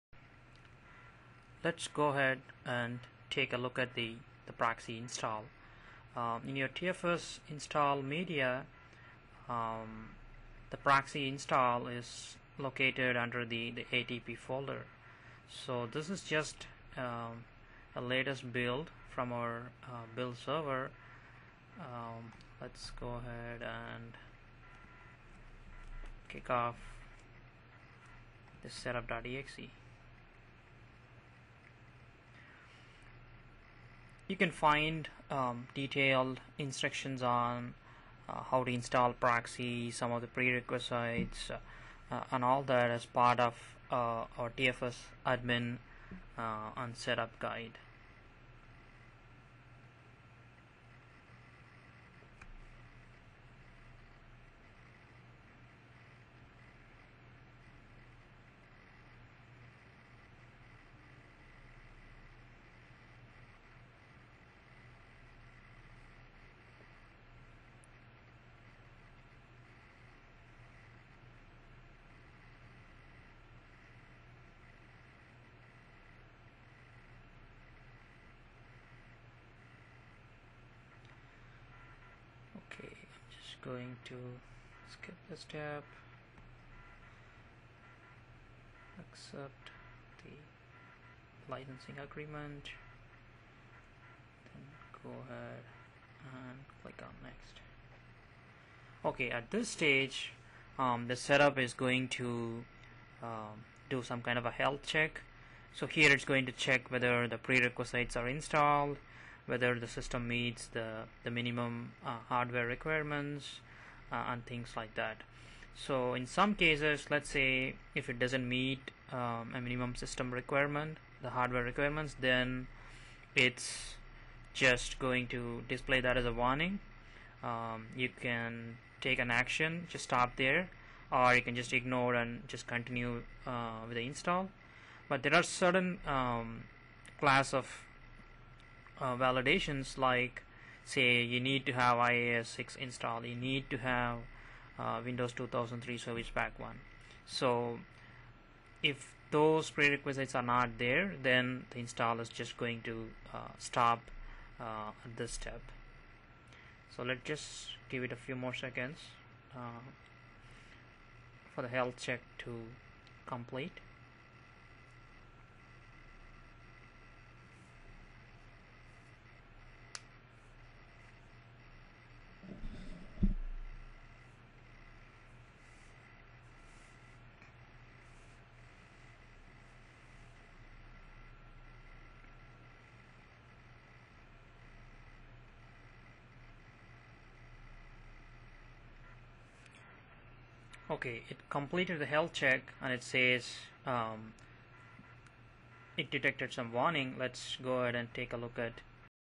TFS Proxy Configuration Part 1 of 4
TFS Proxy Video Tutorial help in configuring TFS Server on a remote server